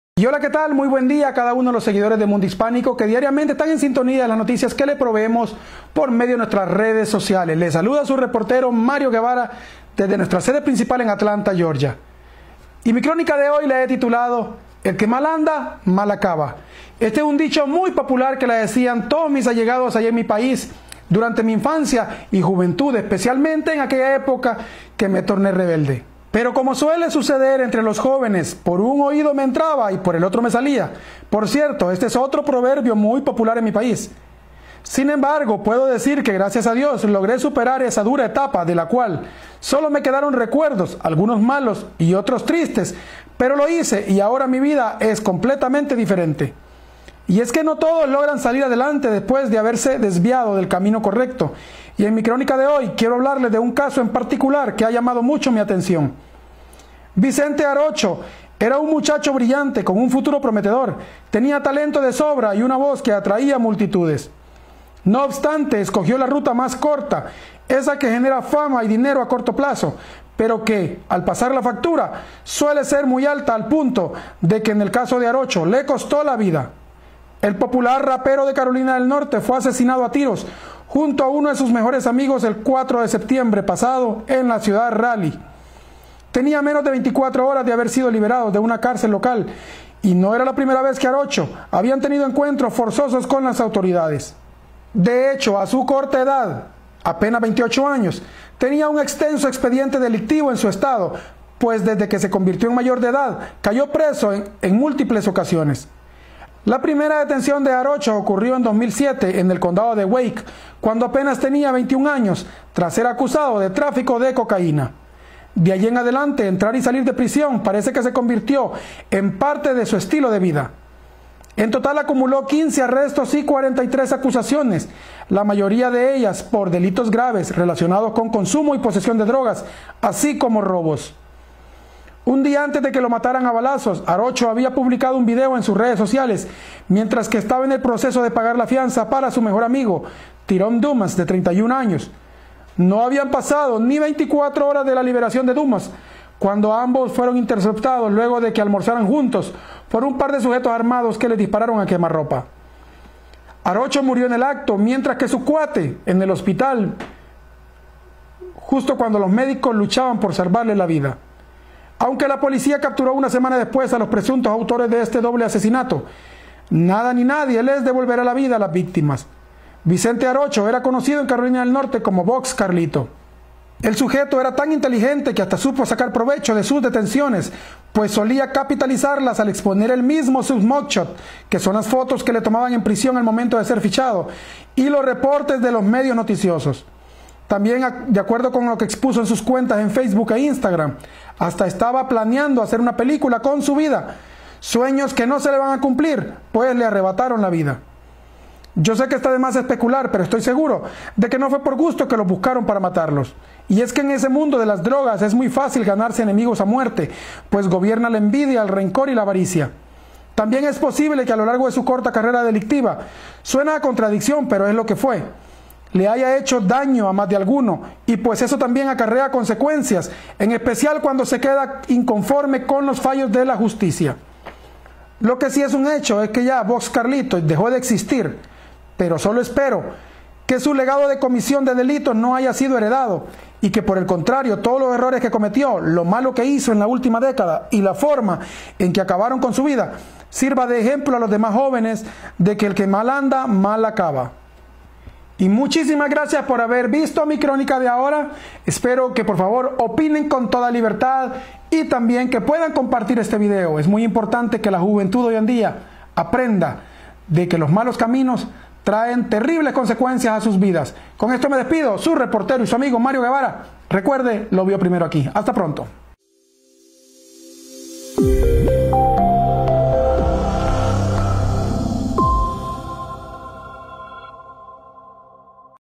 Conocido rapero hispano fue asesinado a tiros junto a su mejor amigo
Vicente Arocho, un popular rapero de Carolina del Norte fue asesinado a tiros junto a uno de sus mejores amigos el pasado 4 de septiembre en la ciudad de Raleigh.

Arocho contaba con un expediente delictivo en su estado, ya que desde que cumplió la mayoría de edad, resultó preso en varias ocasiones, la mayoría de los delitos graves relacionados fueron por consumo y posesión de drogas y robos.